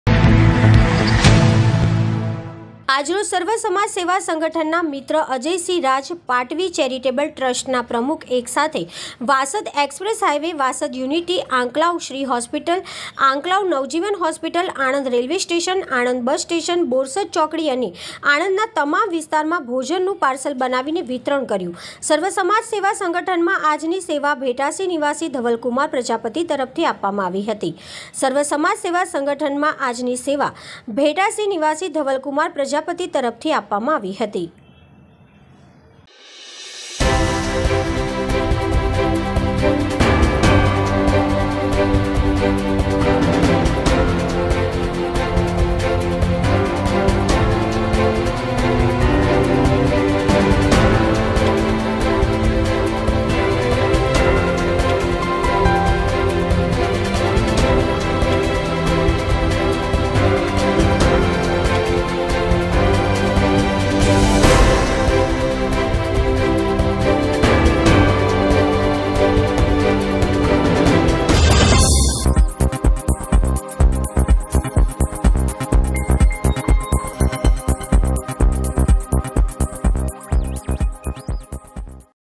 સર્વ સમાજ સેવા સંગઠનમાં આજની સેવા ભેટાસી નિવાસી ધવલ કુમાર પ્રજાપતિ તરફથી આપવામાં આવી છે
આજરોજ સર્વ સમાજ સેવા સંગઠનના મિત્રો..અજય..સિંહ રાજ
 પાટવી ચેરી  ટેબલ ટ્રસ્ટ પ્રમુખ

એક સાથે વાસદ એક્સપ્રેસ હાઈવે, વાસદ યુનિટી ,આકલાવ શ્રી હોસ્પિટલ, આંકલાવ નવજીવનહોસ્પિટલ,આણંદ રેલવે સ્ટેશન, આણંદ બસસ્ટેશન,બોરસદ ચોકડી, અને આણંદના તમામવિસ્તારોમાં ભોજન નું પાર્સલ બનાવીને વિતરણ કર્યું
સર્વ સમાજ સેવા સંગઠનમાં આજની સેવા ભેટાસી નિવાસી ધવલ કુમાર પ્રજાપતિ તરફથી આપવામાં આવી

 પ્રેસ રિપોર્ટર યુસુફ રાજ